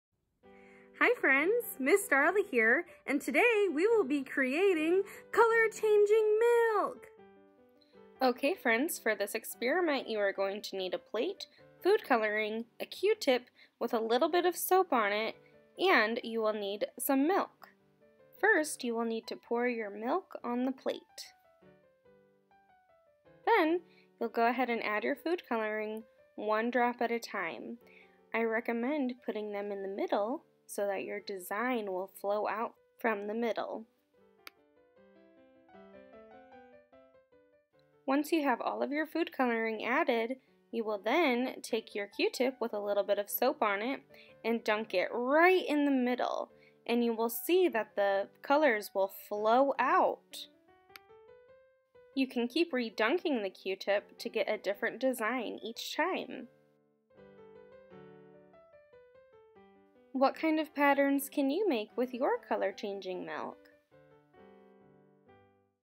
DIY Science: Color Changing Milk
Enjoy a fun color changing milk experiment with Miss Darla, Youth Assistant at Centre Township Branch.